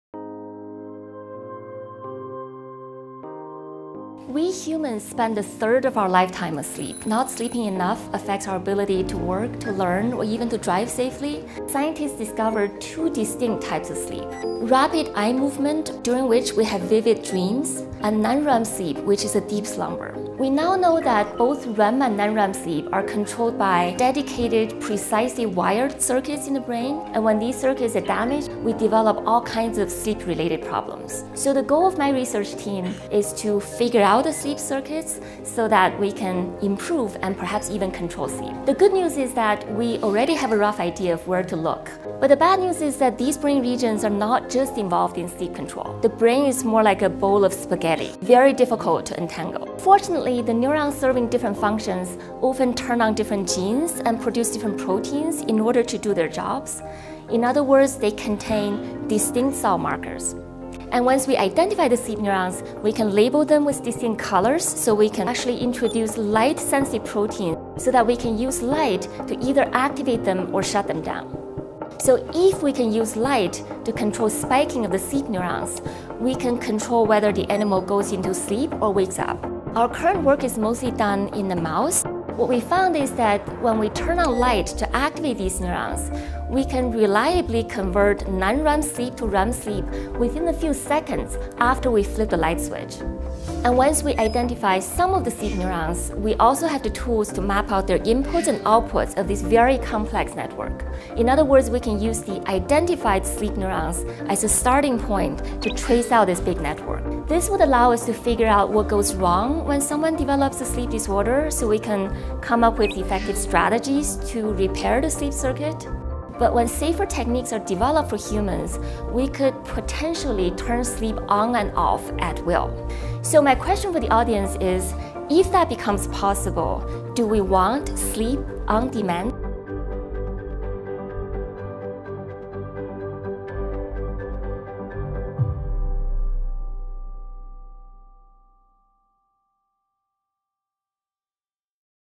Falling asleep, as easy as flipping a switch?
Inadequate sleep is related to a wide range of problems, from depression to obesity. Dan Yang, Professor of Neurobiology, discusses our increasing understanding of the neuronal circuits involved in sleep.  One day, it could become possible to control these circuits.

Please help us grow by sharing.